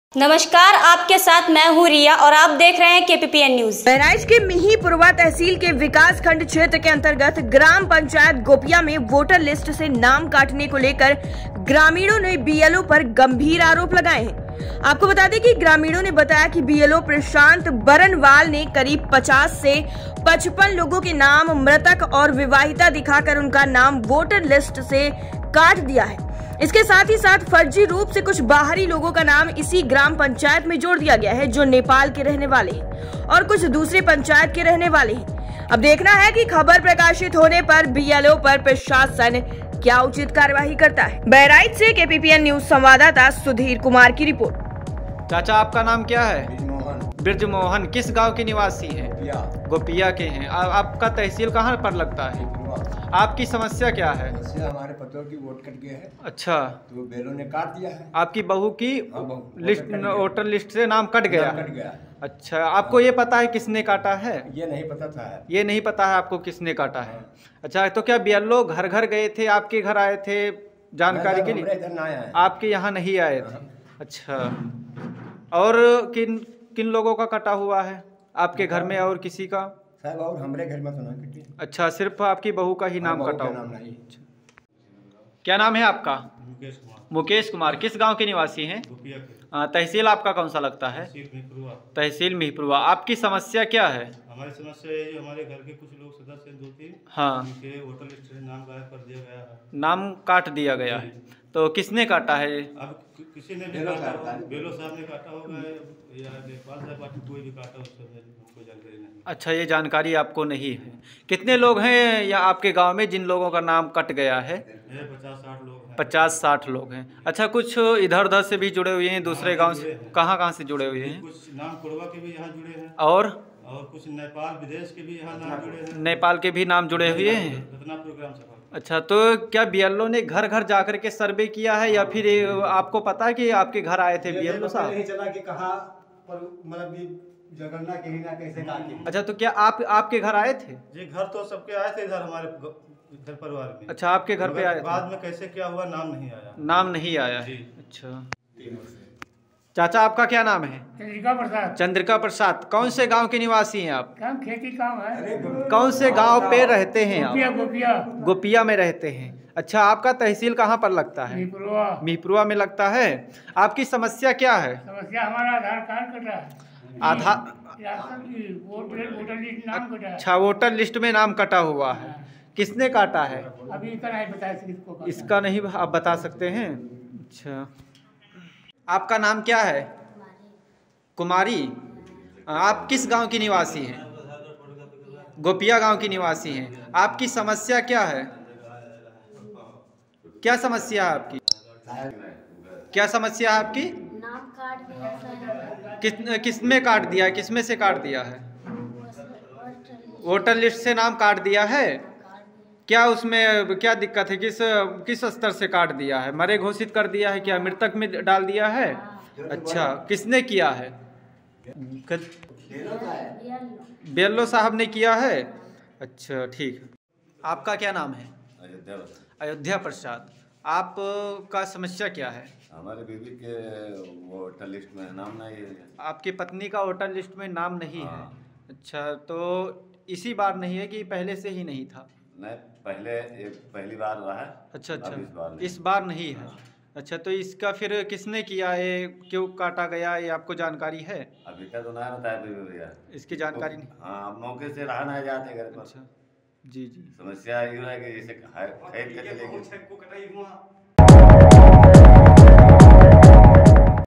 मतदाता सूची से नाम हटाने को लेकर ग्रामीणों ने बीएलओ पर लगाया आरोप.....KPPN NEWS LIVE
मतदाता सूची से नाम हटाने को लेकर ग्रामीणों ने बीएलओ पर लगाया आरोप
सुधीर कुमार
बहराइच